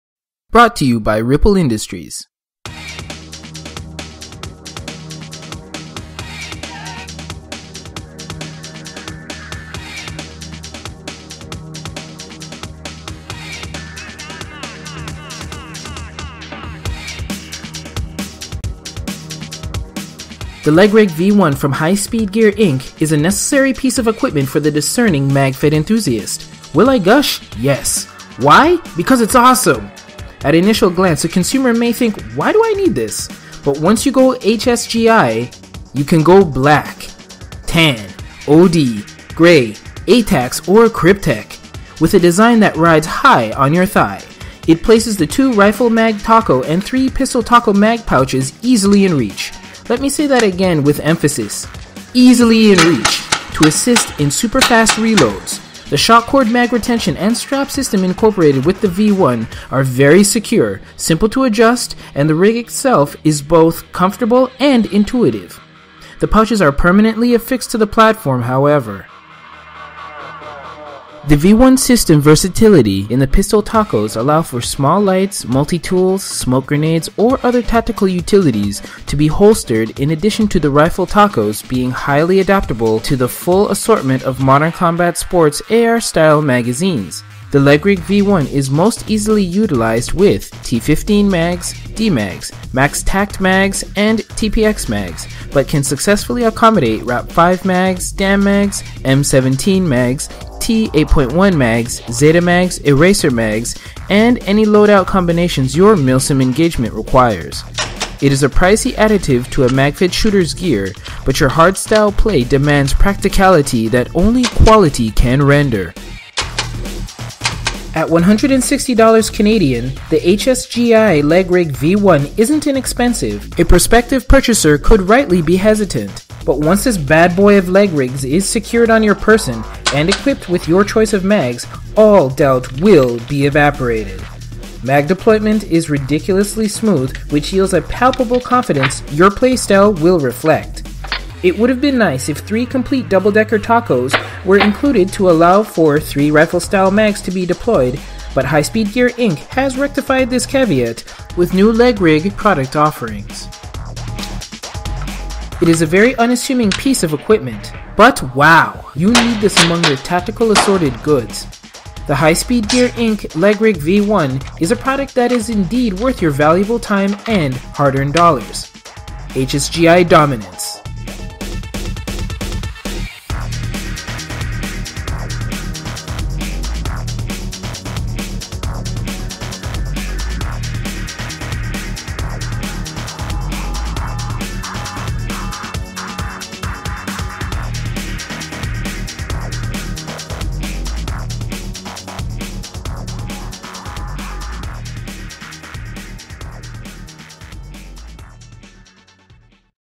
RapidShotz GearShotz - HSGI Leg Rig V1
The HSGI Leg Rig V1, is a product that is indeed worth your valuable time and hard earned dollars!
Les jambe sachet V1 de HSGI, est un produit qui est bien utile de votre temps précieux et argent durement gagné!

CONFIGURATION
(2) Double Decker TACOS, (1) Pistol TACO

MASS (g)
469 g (16.5 oz)

DIMENSIONS (cm, L x H x W)
22.7 x 14.0 x 7.2 cm

PRICE (CDN) – at time of publication
$160.00